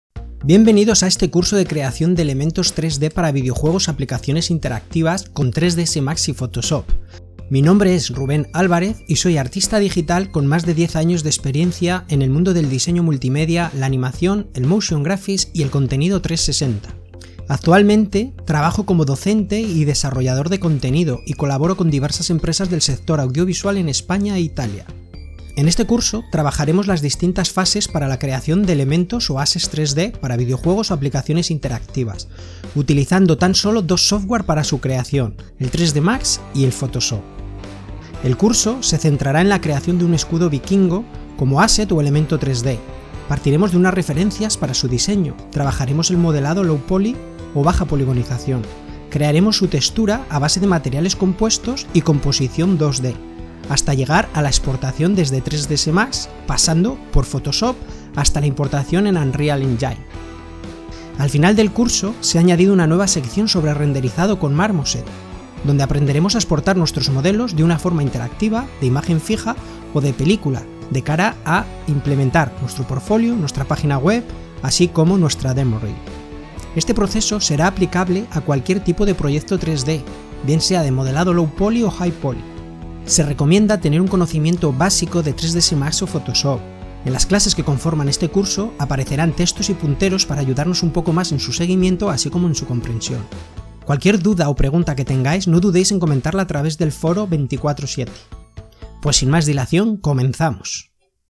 Técnicas para la creación de elementos 3D para Videojuegos
Se ha añadido una nueva sección en la que renderizaremos nuestra geometría en Marmoset Toolbag para conseguir un acabado aun más atractivo para una exportación fija, interactiva y para vídeo

¿Qué aprenderán los estudiantes en tu curso?

Creación de elementos o asset 3d para videojuegos o aplicaciones interactivas con 3ds Max y Photoshop. Presentación y Renderizado con Marmoset.

En base al proyecto "Escudo Vikingo" abordaremos las fases de : Modelado en Baja Poligonización ( Low Poly),  Coordenadas de Mapeado, Creación de IDs, Materiales Simples, Materiales Compuestos,  Tostado de Texturas ( Baked), Composición de Mapas 2D, Exportación FBX ( Game Exporter ), Importación en Unreal Engine.

¿Hay requisitos para realizar el curso?

Conocimientos básicos 3ds Max y Photoshop

Descripción:
En este pequeño curso trabajaremos de una forma sencilla el proceso completo (workflow) para crear “assets o elementos 3D” para videojuegos o aplicaciones interactivas, usando dos de las herramientas más estandarizas de la industria en la creación de contenido 3D; Autodesk 3ds Max y Adobe Photoshop.

Trabajaremos en torno al diseño de un “Escudo Vikingo” pasando por todas sus fases, el modelado Low Poly, el despliegue de coordenadas UV para proyectar la textura, la creación de mapas simples y mapas compuestos (Blend), el tostado de mapas (diffuse, normal, AO), la composición 2D y el añadido de detalles, la exportación del modelo e importación y comportamiento en Unreal Engine.

Este workflow será aplicable a cualquier tipo de proyecto de creación de contenido 3D (LowPoly o HighPoly )